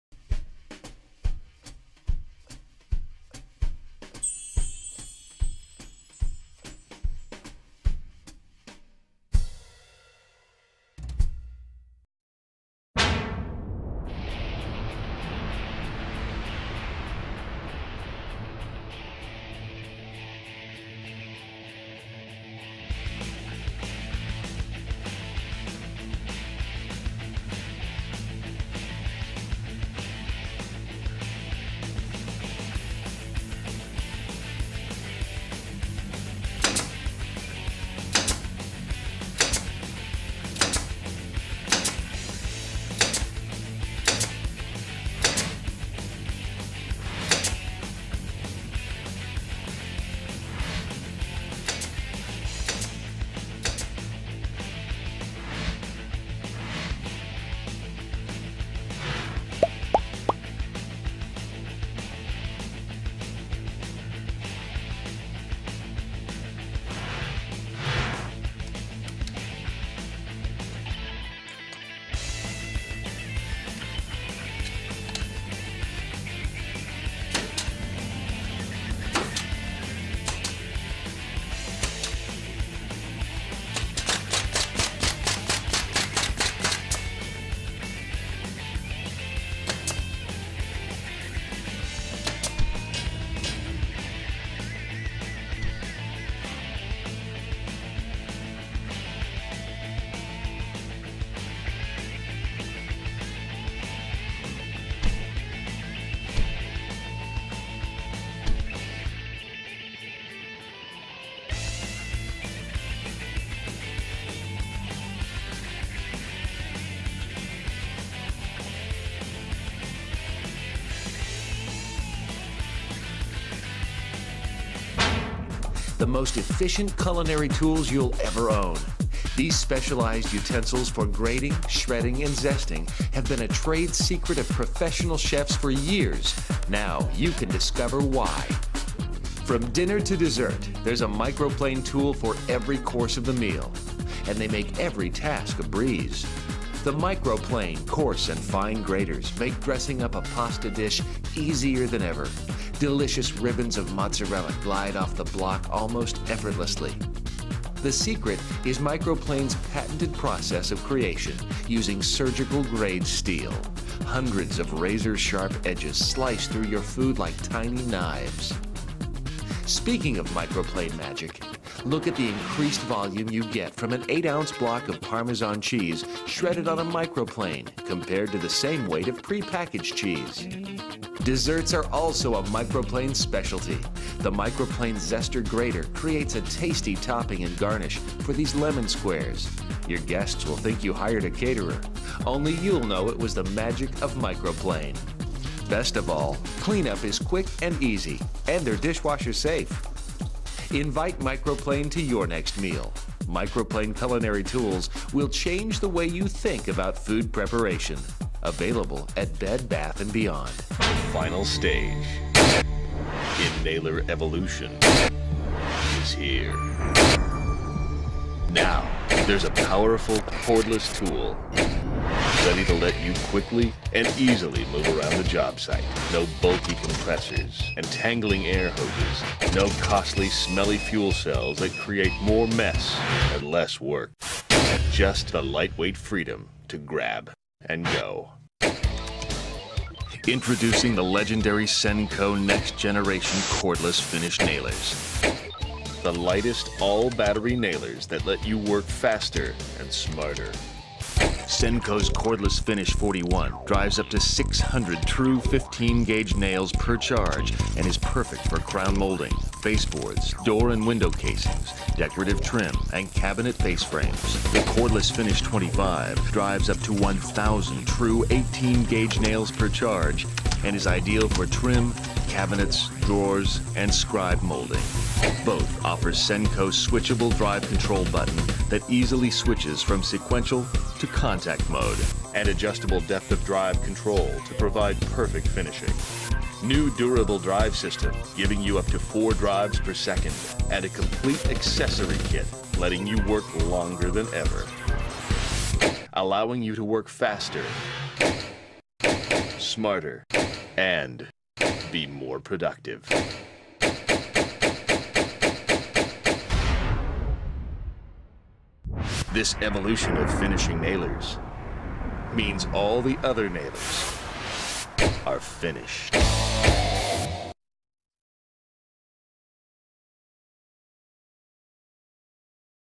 Point of Purchase Video Demo
Black Box Media Group designed these videos for clients who wanted to utilize the latest technology in product promotion. Video screens displaying these pieces were set up close to the product displays in stores. The results speak for themselves.